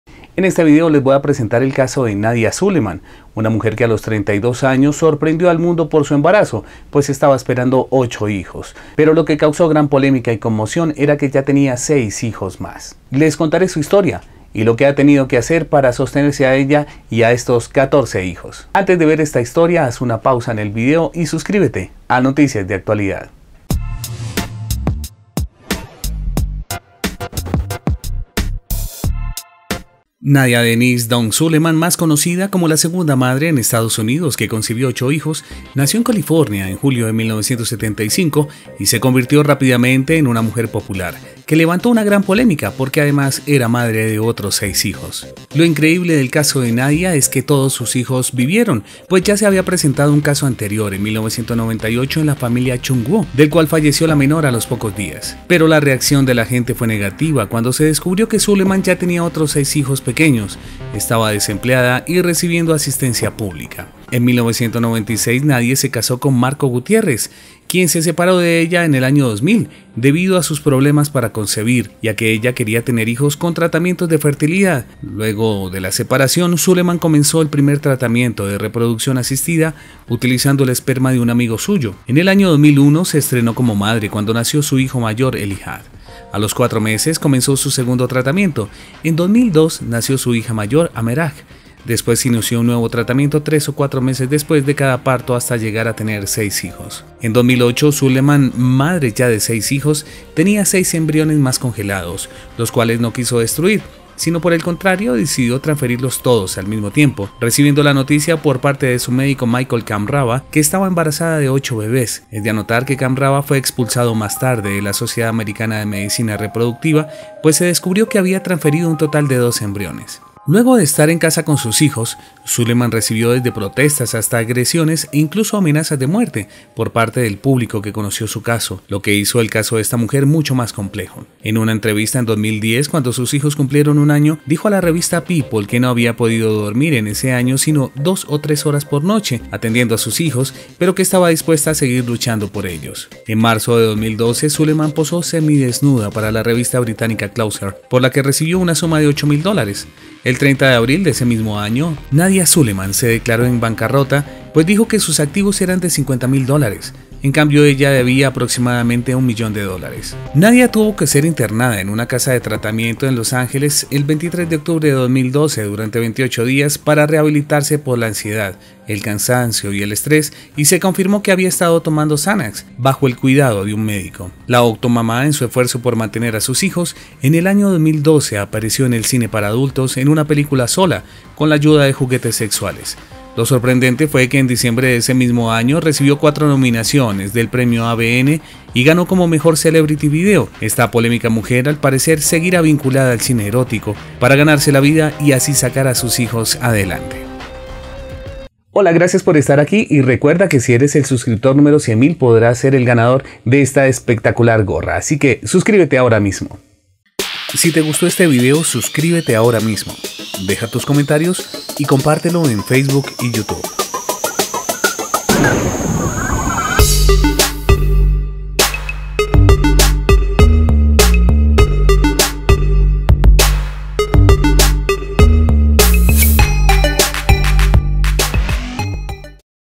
Tuvo 8 Bebés en un solo Embarazo y completó 14 Hijos en Total / Nadya Suleman la Octomamá
Nadya Suleman, es una mujer californiana que dio a luz a ocho hijos en un solo parto, y que completó en total 14 hijos. Conozca la interesante y turbulenta historia de esta mujer, que sola ha sacado sus hijos adelante.

Si te gustó el video dale "me gusta" y compártelo con tus amigos en Facebook o Twitter, de verdad nos ayudarías mucho.